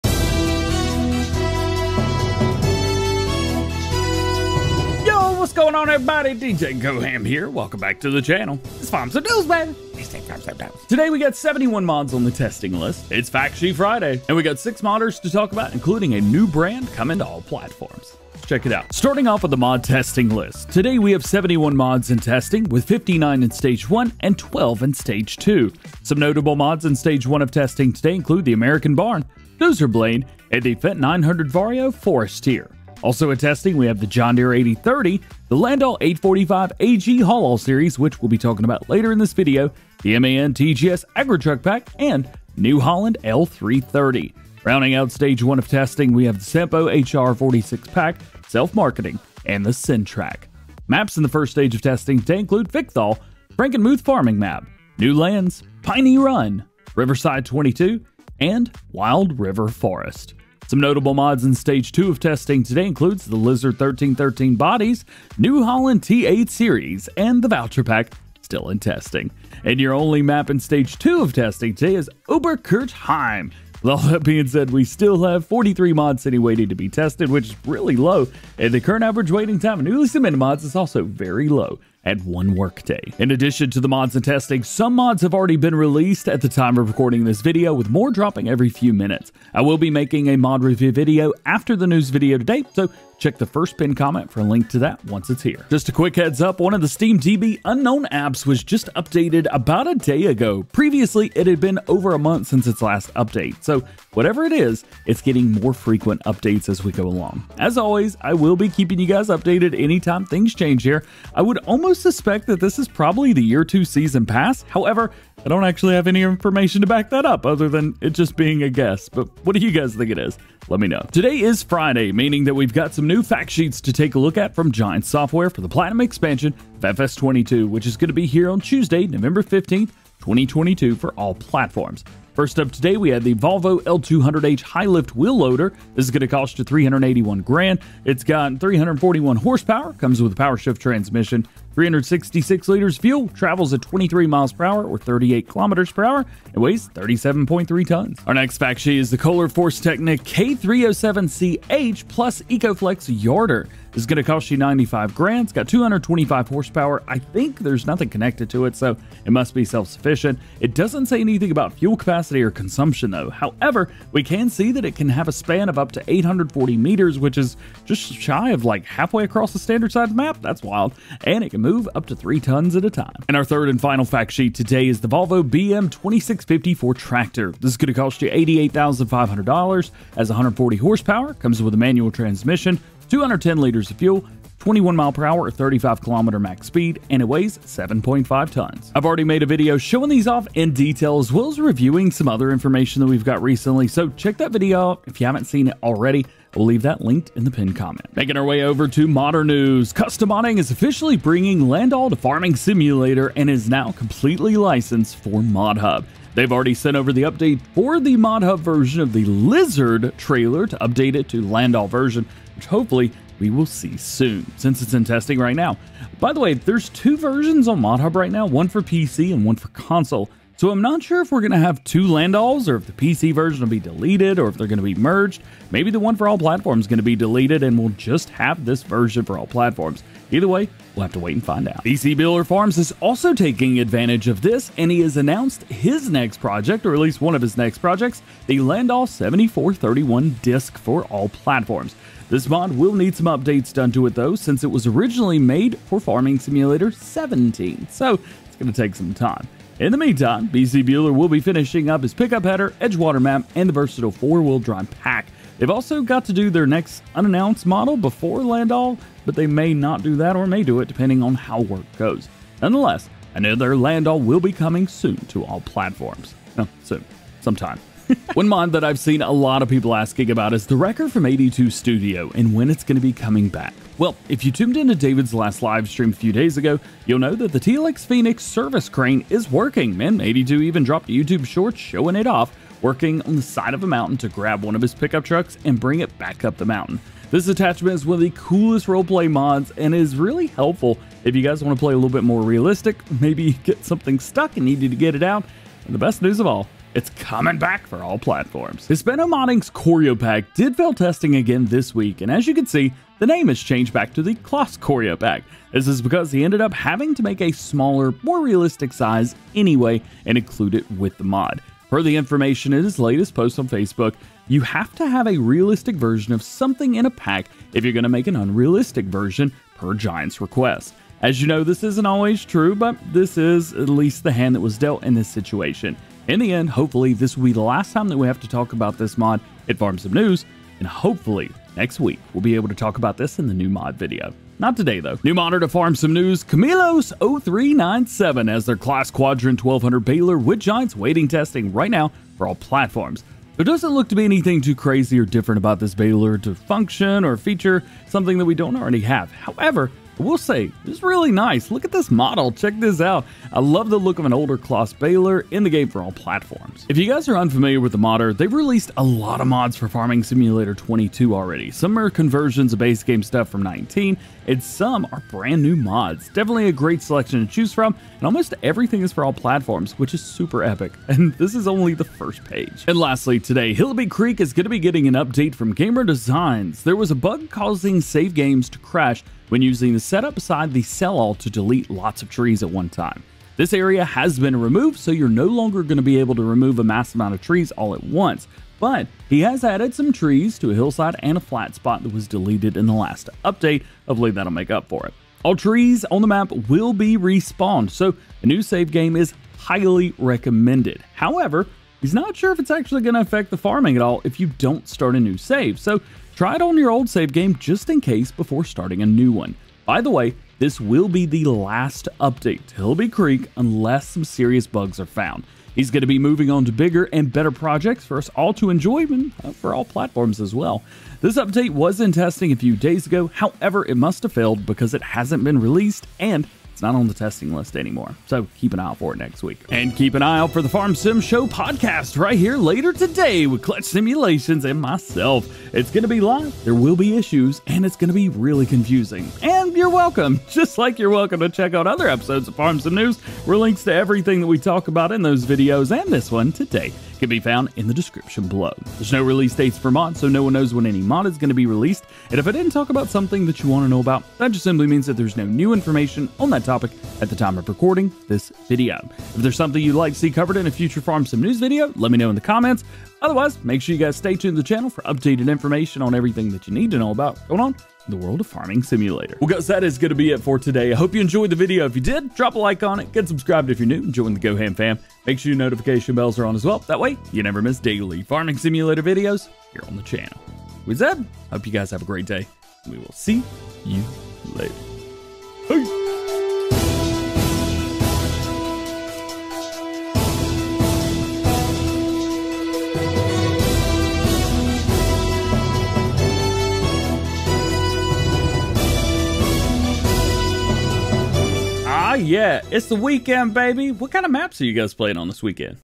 Farm Sim News! TLX Wrecker, Landoll For All, & Claas Mods! | Farming Simulator 22
Farm Sim News! TLX Wrecker, Landoll For All, & Claas Mods!

0:00 - Intro
0:11 - Top Stories
0:18 - Mods In Testing
1:23 - New Mods Out Now
1:35 - SteamDB Update
2:01 - Fact Sheet Friday
3:18 - Landoll Is Licensed!
3:51 - New Landoll Mod
4:28 - TLX Wrecker Is Back
4:58 - It's A Claas Again
5:35 - Claas Quadrant 1200
6:12 - Hillibee Creek Update
7:09 - Segue & Disclaimer
7:49 - Outro
8:21 - What map this weekend?